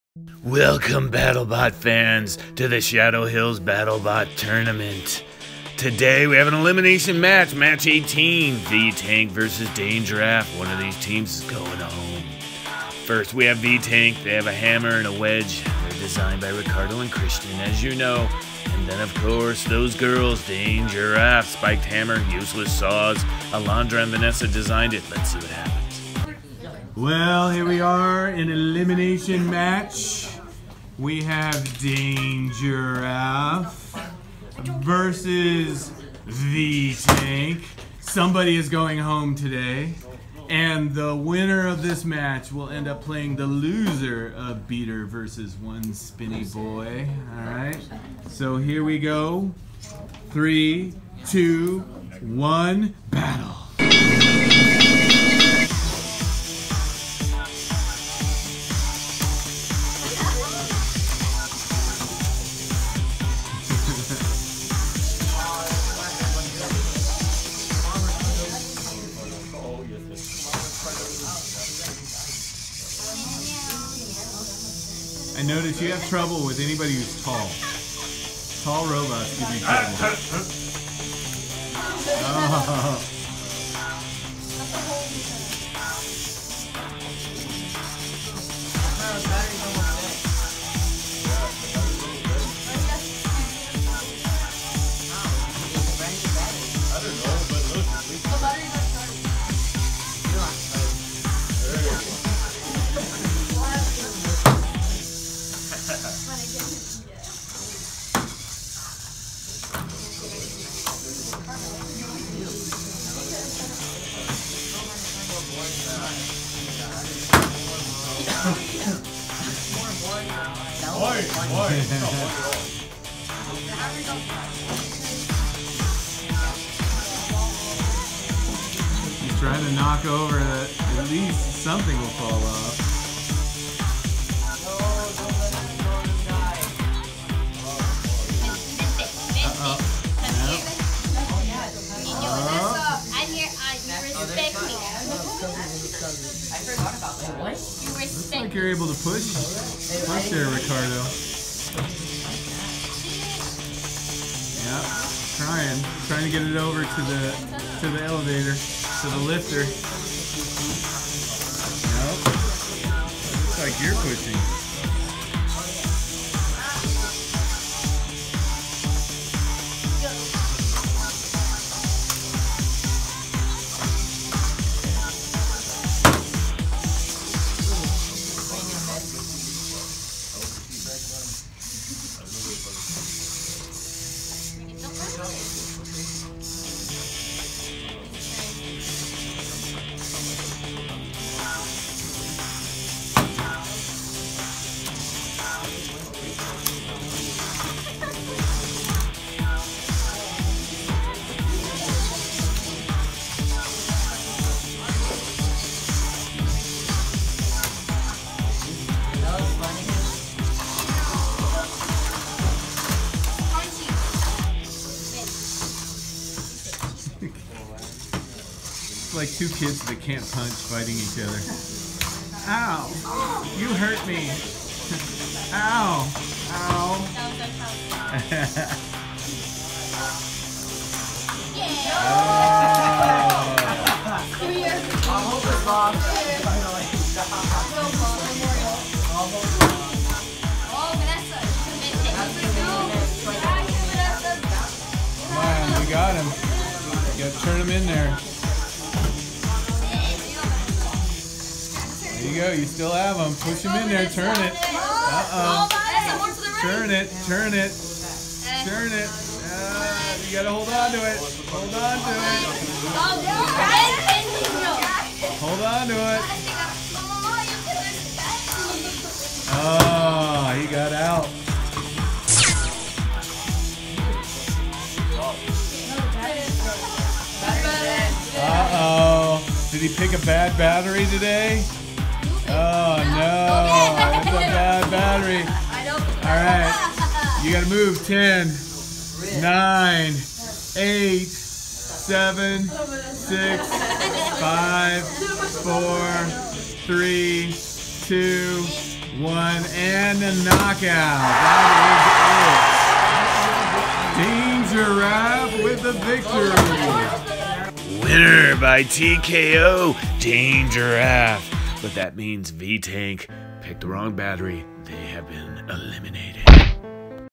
2018 Shadow Hills VEX IQ Battlebot Tournament Match #18
Today’s elimination match pits Dane Giraffe vs. V-Tank. The tournament ends for one of these teams today.